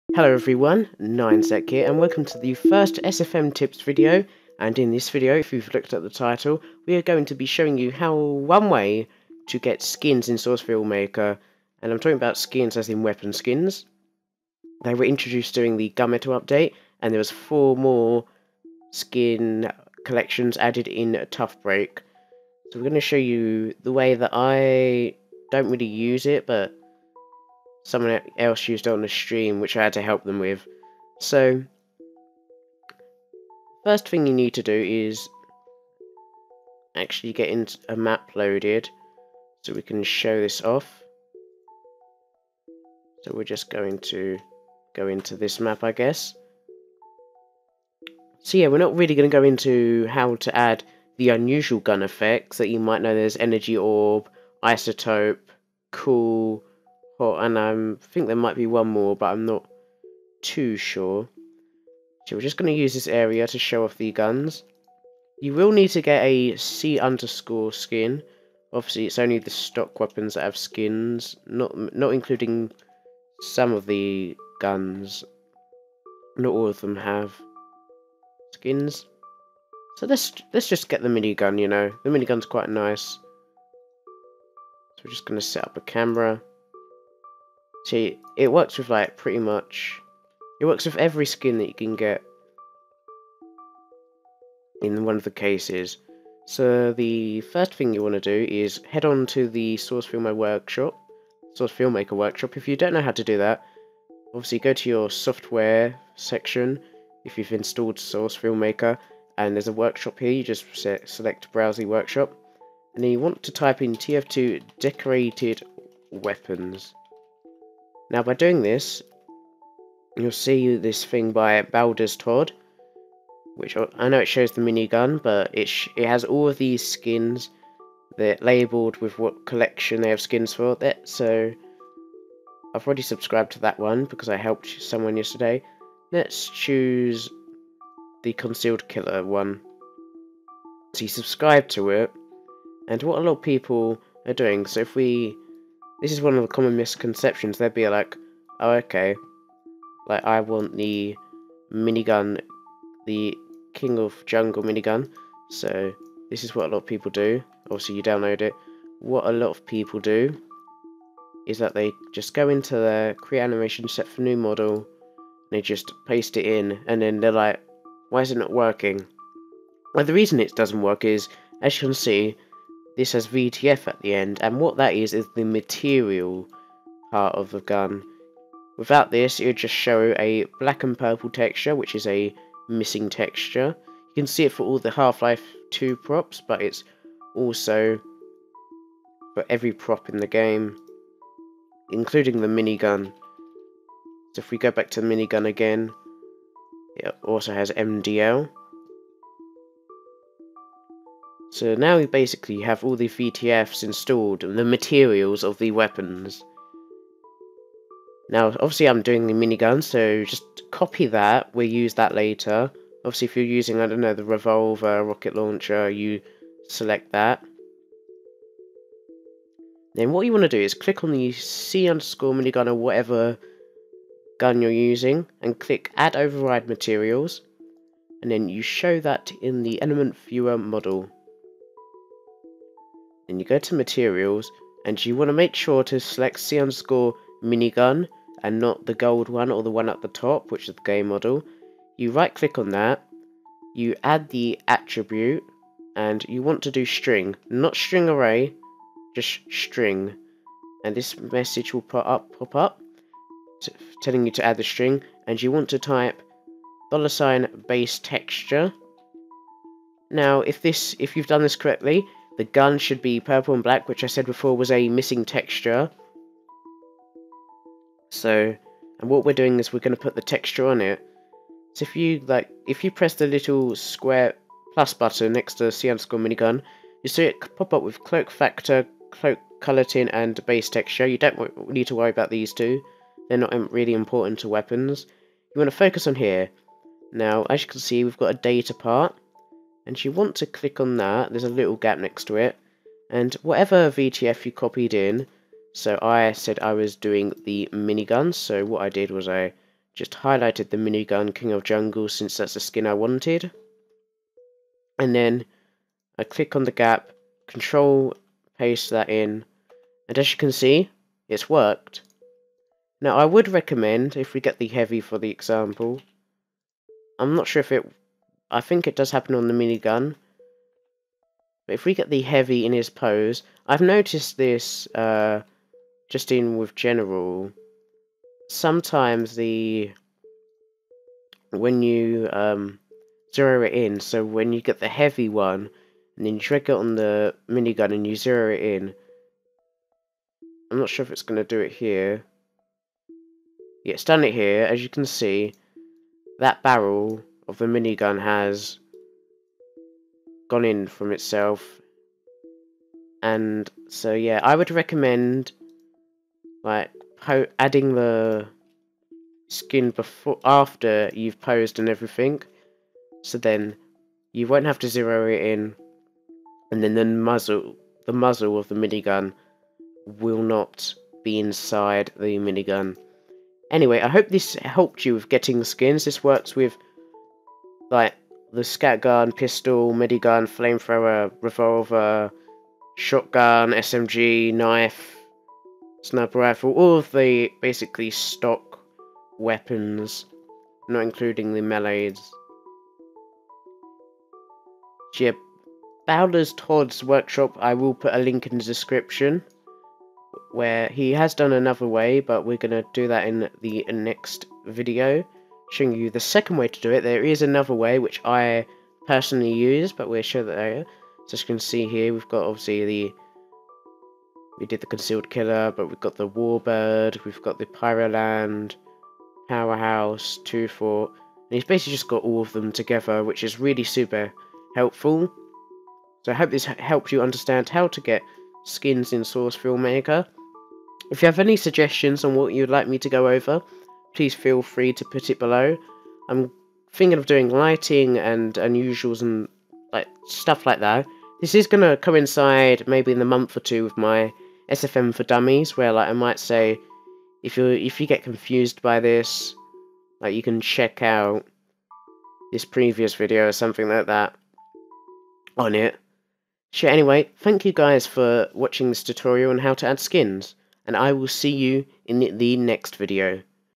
SFM Tips #1 : How To Get Weapons Skins In SFM (1st Way)
Hello NyanZak here,  with the start of the brand new series, SFM Tips. Where I show you some useful things in SourceFilmMaker. In this episode I show you one of 3 ways of getting weapon skins in SFM.

Baldurs Tod Skin Items (Page 2 & 3)